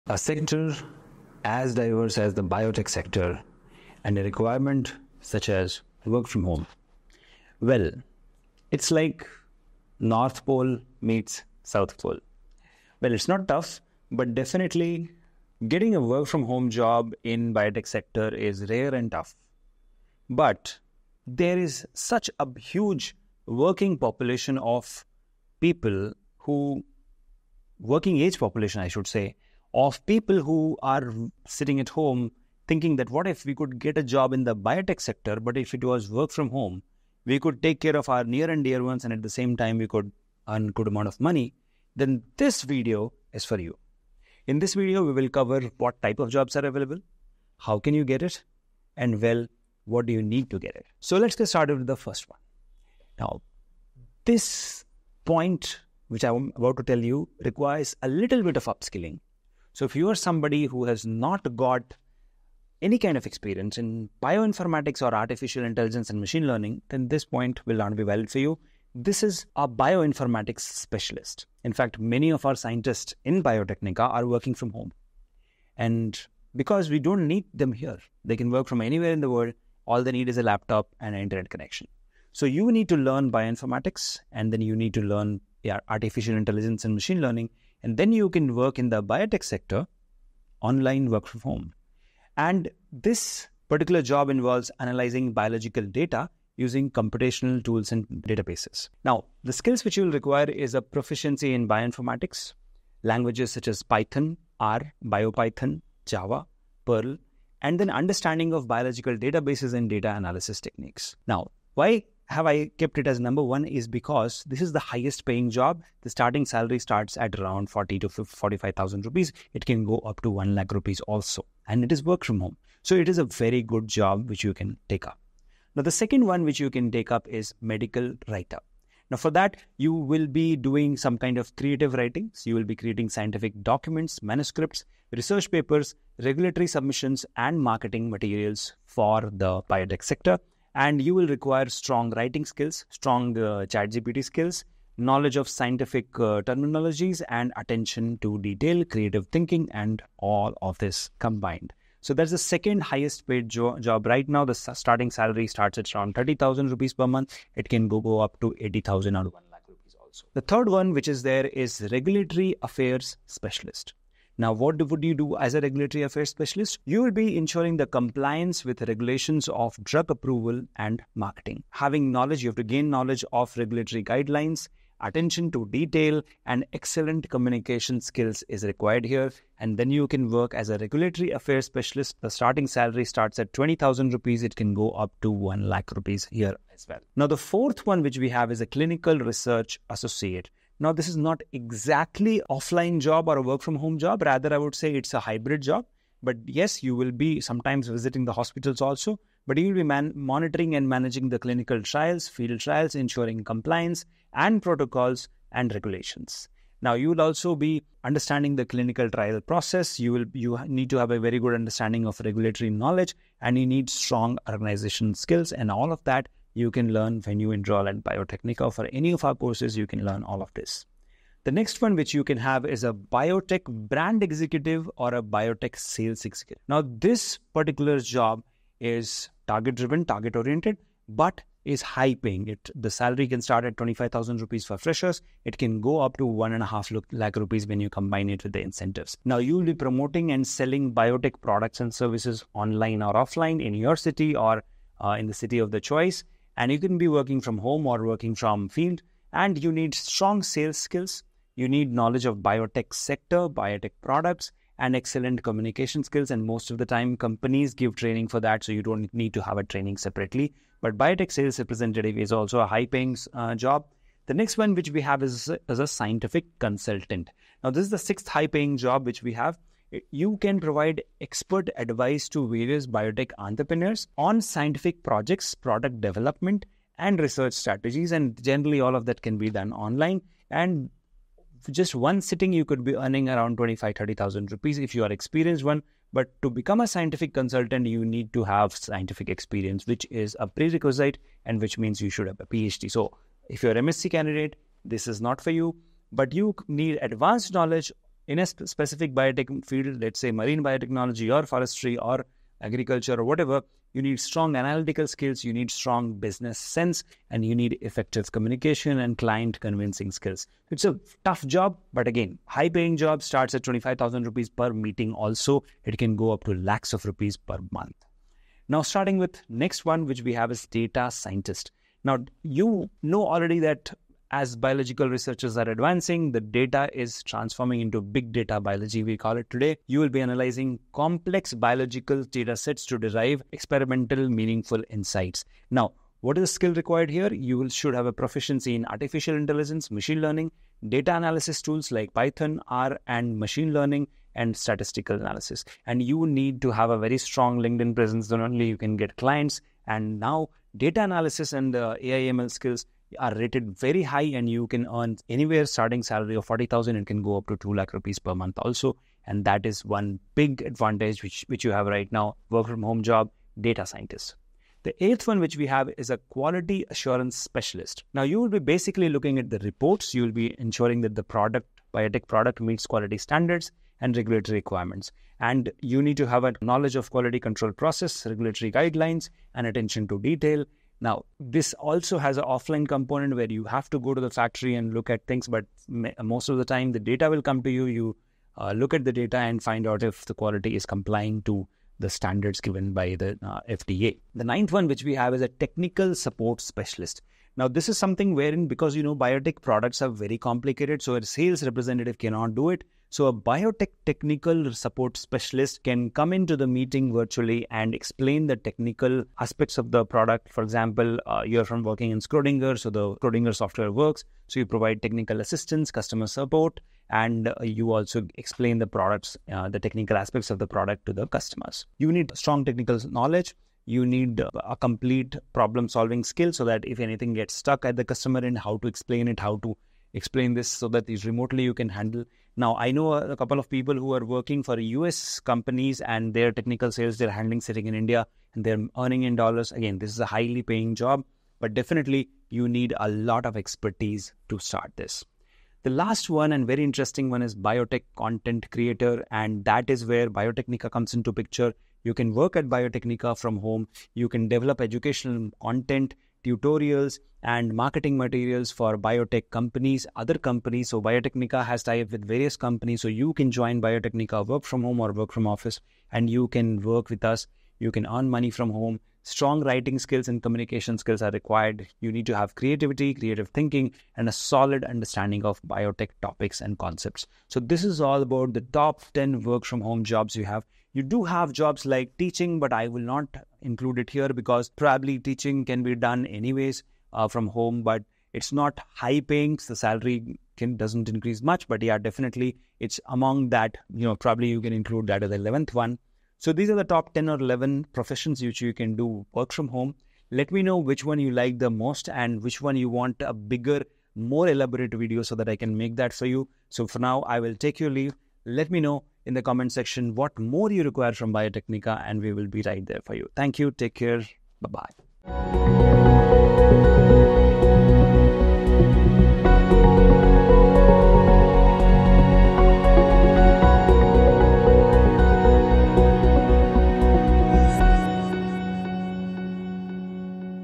TOP 10 Work From Home Biotech Jobs - Exclusive - Must Watch
Discover the best work-from-home opportunities in the biotechnology field! In this exclusive video, we explore the top 10 biotech jobs that allow you to work remotely. From research and development to data analysis, find out which roles are in high demand and how you can kickstart your remote biotech career. Don't miss this must-watch guide for anyone looking to combine their passion for biotech with the flexibility of remote work.

Key Points Covered:

- Overview of top work-from-home biotech jobs
- Skills and qualifications required
- How to apply for these positions
- Benefits of remote work in biotech
- Tips for succeeding in a remote biotech role